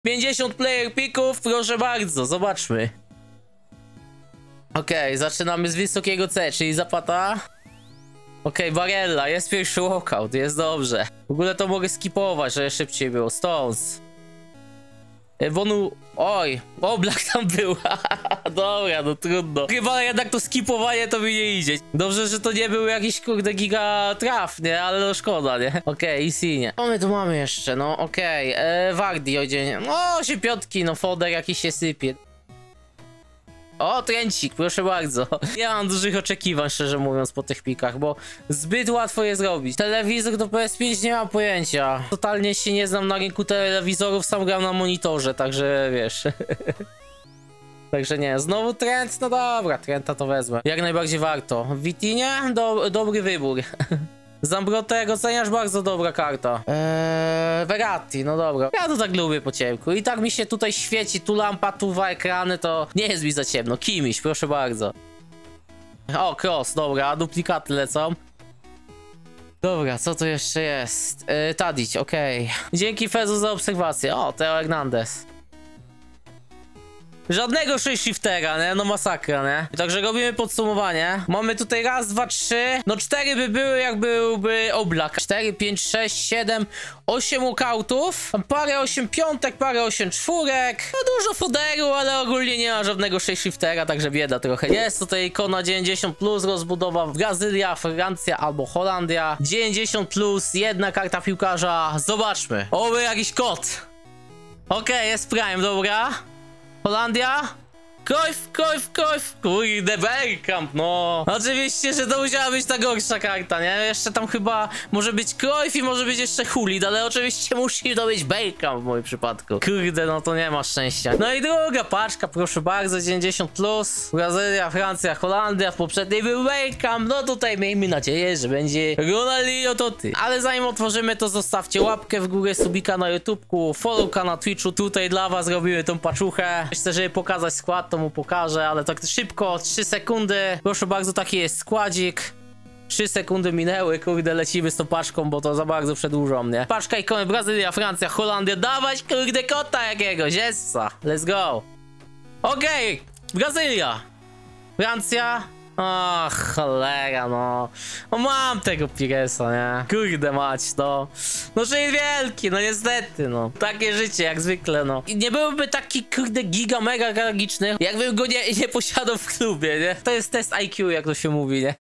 50 PICKÓW 81+ & DWIE IKONY 90+ W WERSJI SPEEDRUN
ZOSTAW SUBA JUŻ TERAZ!

SIEMA, TYM RAZEM SZYBKA AKCJA, NAJLEPSZE Z 50CIU PLAYER PICKÓW 81+ W POSZUKIWANIU ZMIENNOKSZTAŁTNYCH ORAZ DWIE PACZKI Z IKONĄ 90+, GDZIE MOŻNA ZNALEŹĆ IKONY WYŁĄCZNIE Z BRAZYLII, FRANCJI BĄDŹ HOLANDII. FILM W WERSJI *SZYBKA AKCJA*, BO NIE MA SIĘ CO CZAIĆ. MIŁEGO OGLĄDANIA